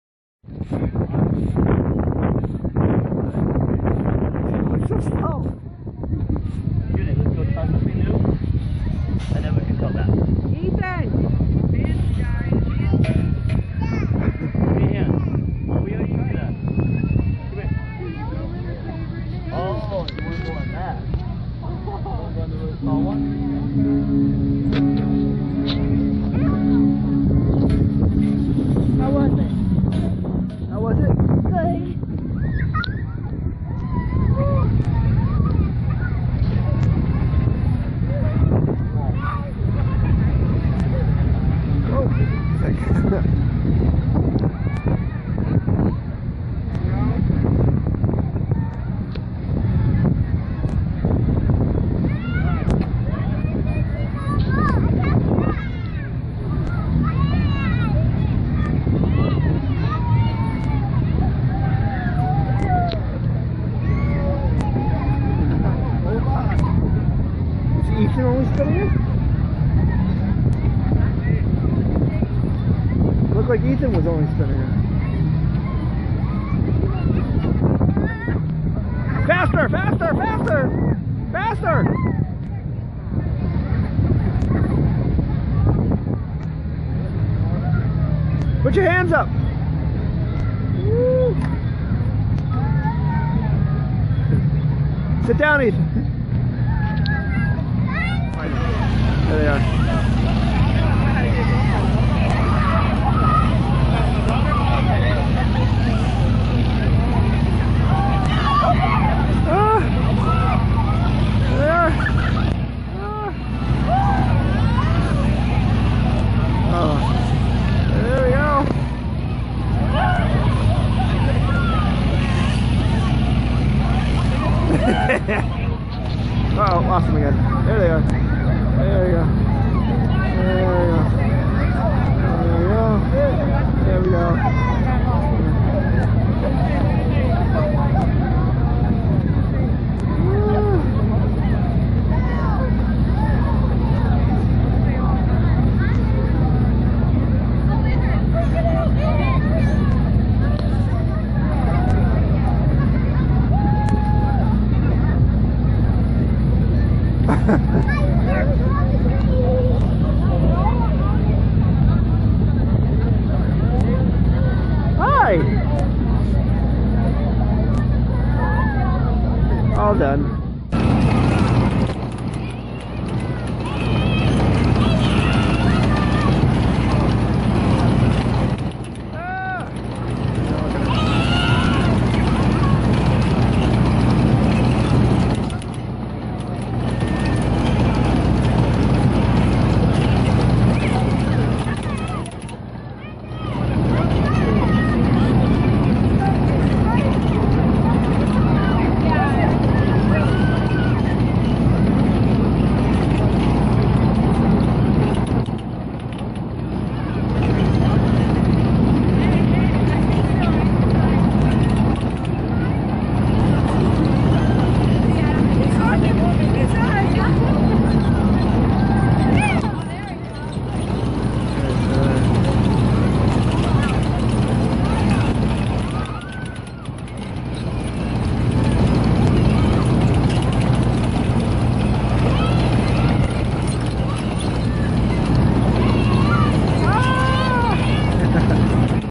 882 - Town Carnival Clips
We took the kids to the local annual town carnival that happens every year around the Fourth of July. Here's a few clips! What you don't see, is Gavin and I going into a haunted house type building, and GAV being scared by an older guy in the dark. I guess that was his only job. After, the guy came out behind us laughing at Gavin. lol ...